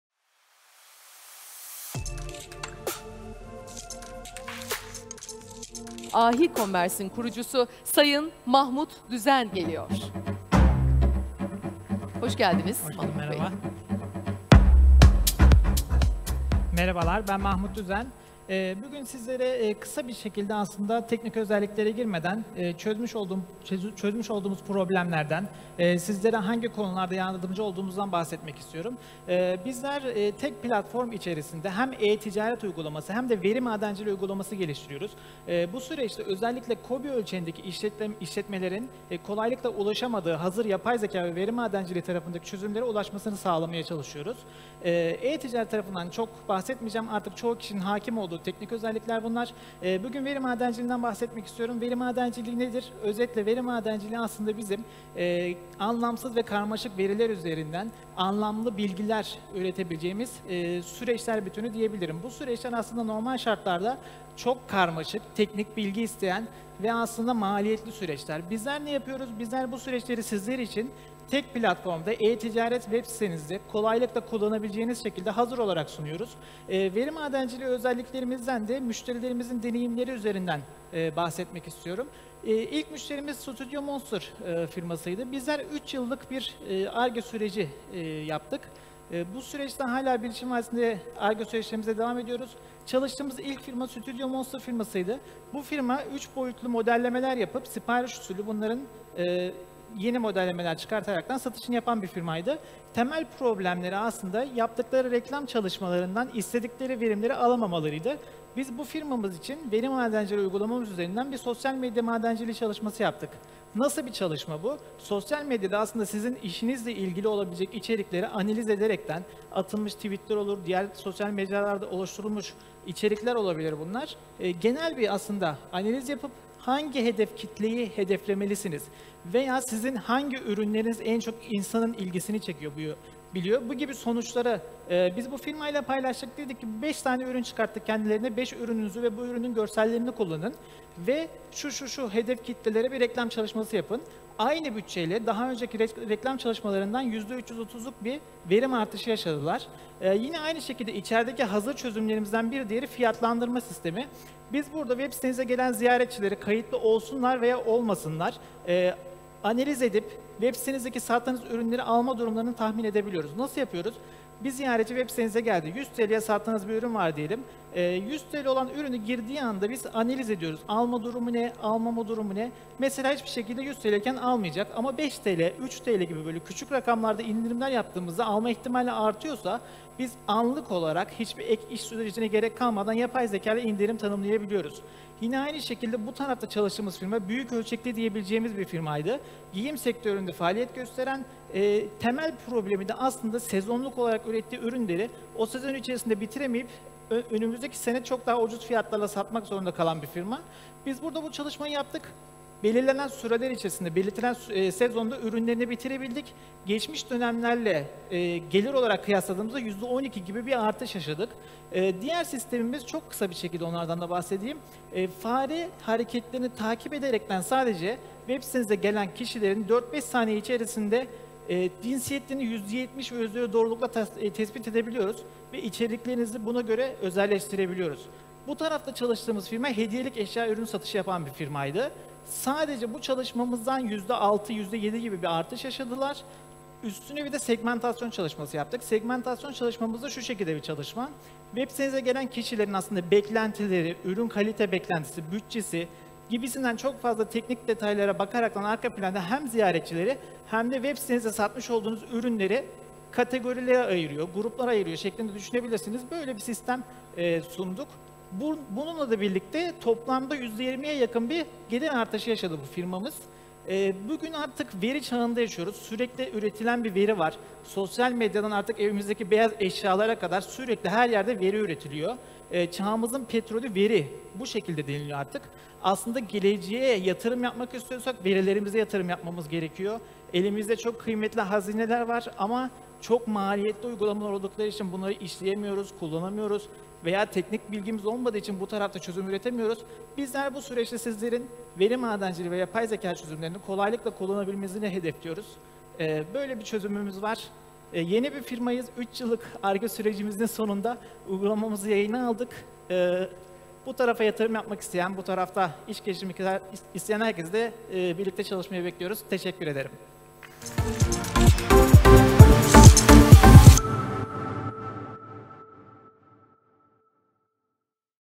Mahmut Düzen - Ahicommerce / Kurucu
12 Ekim 2023’te Selectum City Ataşehir'de Türkiye’nin en yenilikçi etkinliği Oniki’nin on birinci E-Ticaret ve E-İhracat Konferansı’nı gerçekleştirdik.

Geliştirdiğimiz yapay zeka destekli hibrit event teknolojisini yenilikçi konseptimizle buluşturduk ve E-Ticaret ve E-İhracat ekosisteminin en önemli aktörlerini maksimum fayda odağında fiziksel ve online olarak bir araya getirdik!

Etkinliğimizde konuşmacılarımız arasında yer alan Mahmut Düzen - Ahicommerce / Kurucu'nun konuşmasını sizlerle paylaşıyoruz. 🤩

Networking alanında devrim yaratan Oniki etkinliklerinde görüşmek dileğiyle.